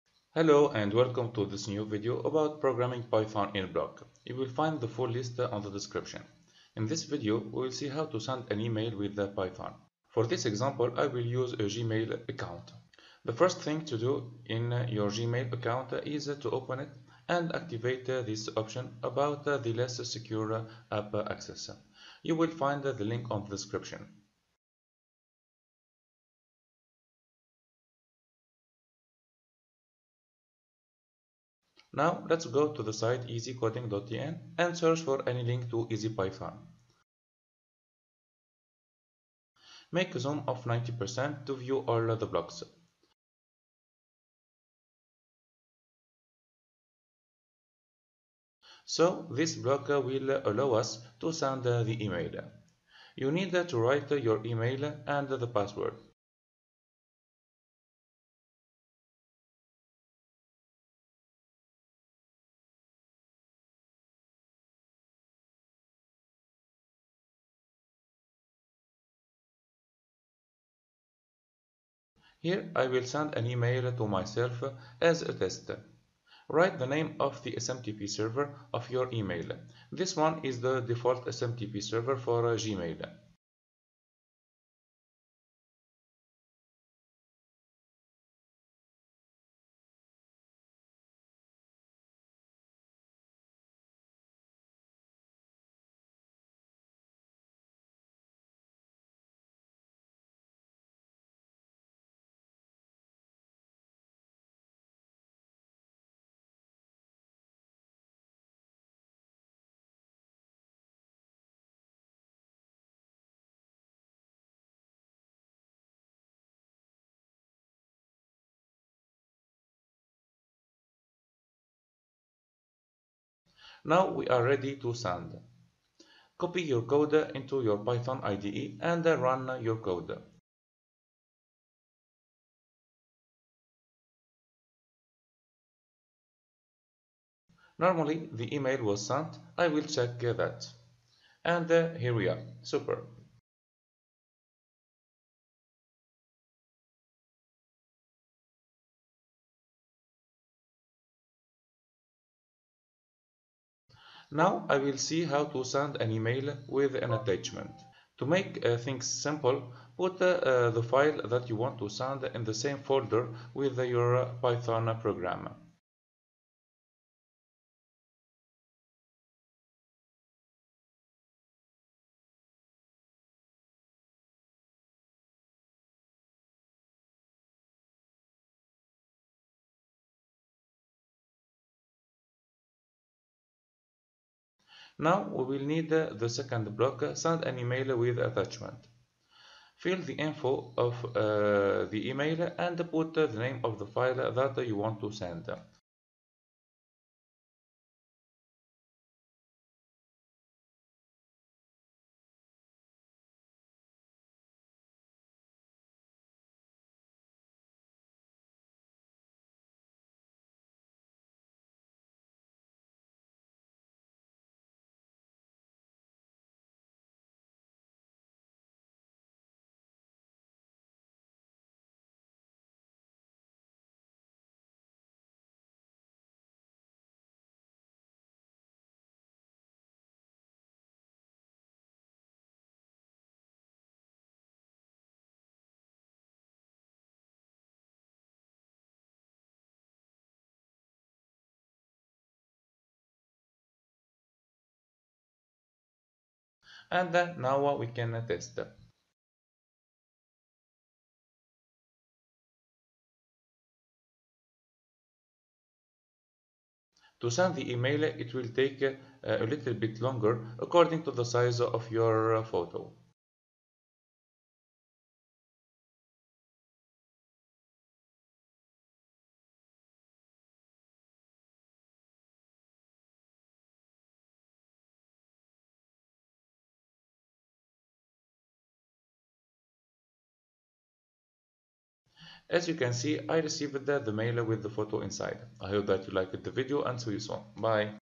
Easy Python tutorial: Send an email
In this video, we will see how to send an email with Python in an easy way.
We will not write code, just snapping blocs together. It will take few minutes.
We will see also how to add an attachment to the email.